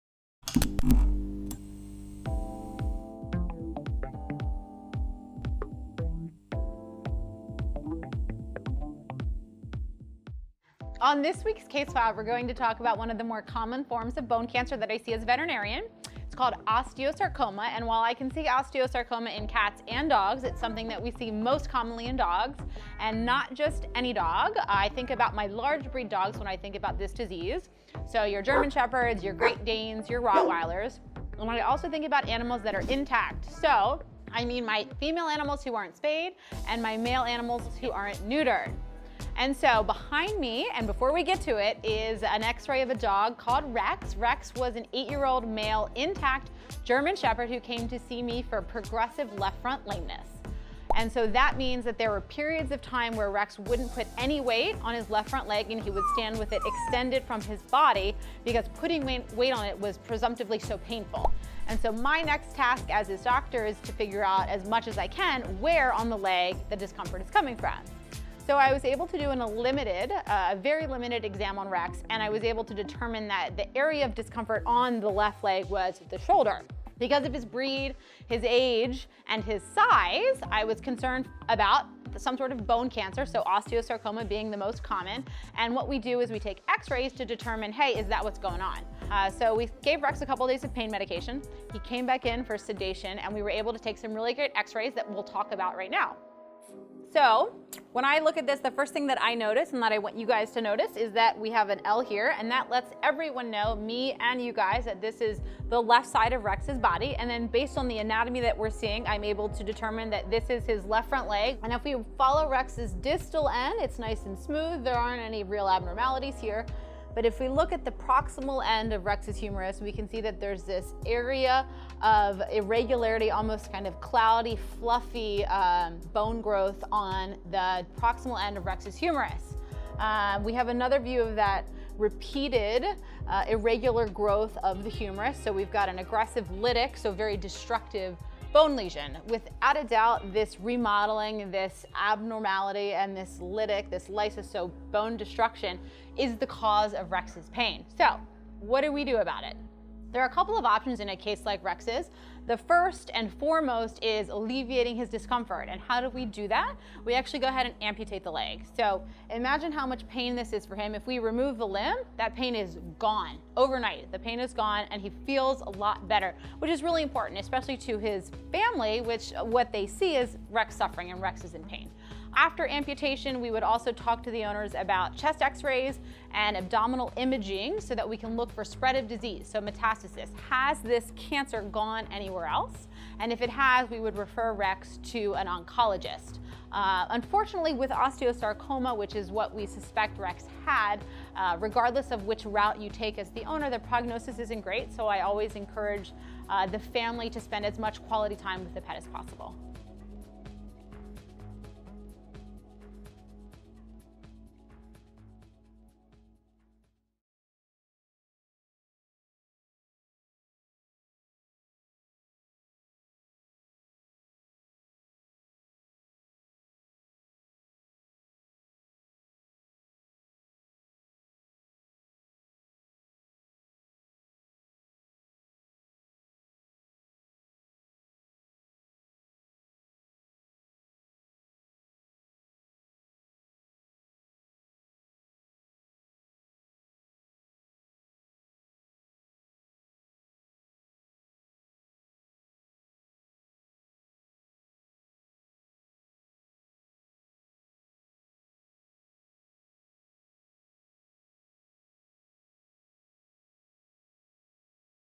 Osteosarcoma is one of the most common forms of bone cancers in dogs, especially in large breeds.
Is your dog limping? Canine bone cancer, osteosarcoma, may be part of the diagnosis.